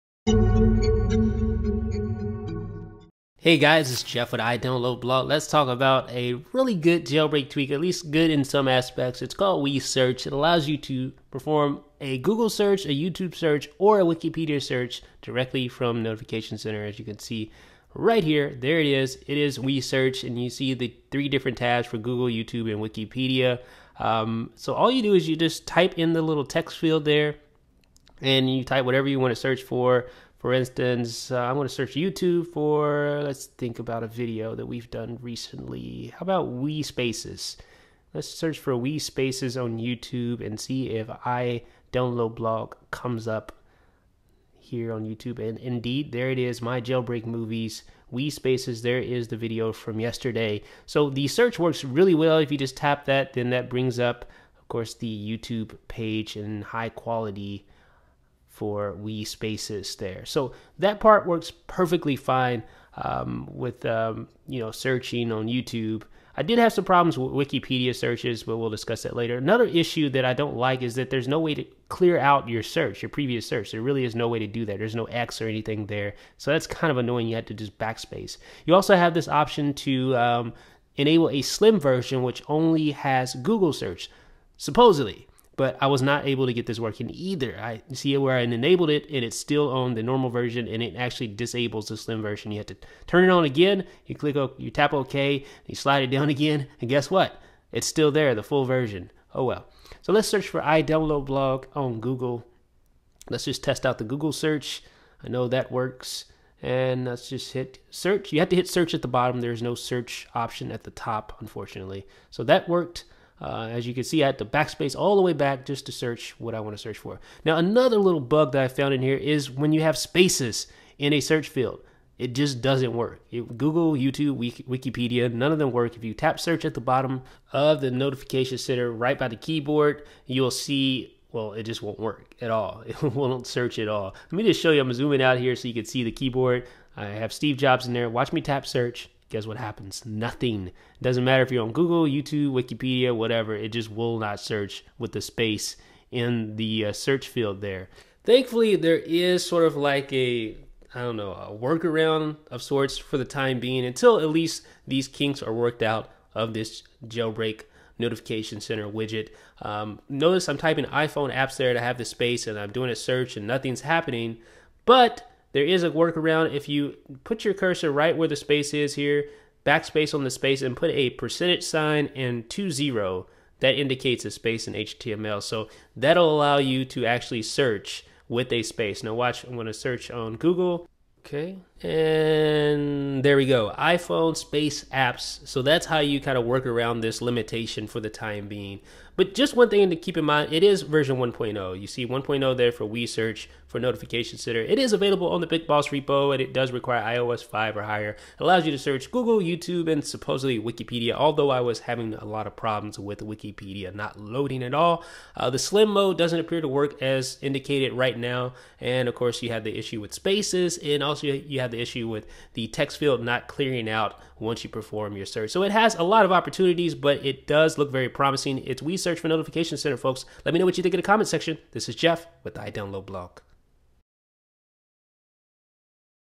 WeeSearch
Name: WeeSearch
Description: Search google, youtube, and wikipedia from Notification Center
Price: Free
Repo: BigBoss
Developer: Jonas Gessner

About iDB